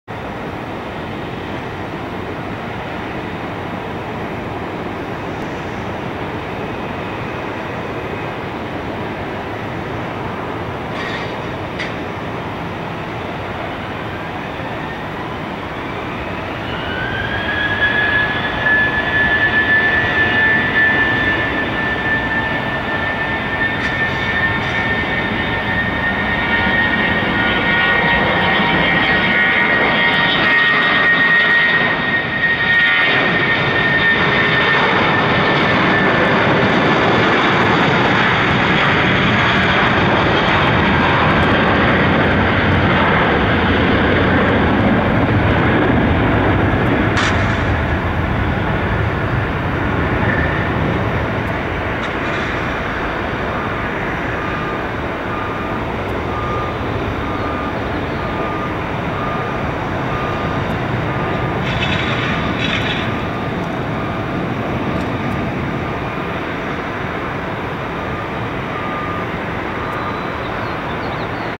Buddy On Board: JetBlue A320 [N796JB] Takeoff Portland Airport (PDX)
Flight: JBU1521 PDX-LGB
Taking off from runway 28L with Rocknroller92 on board.
-Have a nice flight!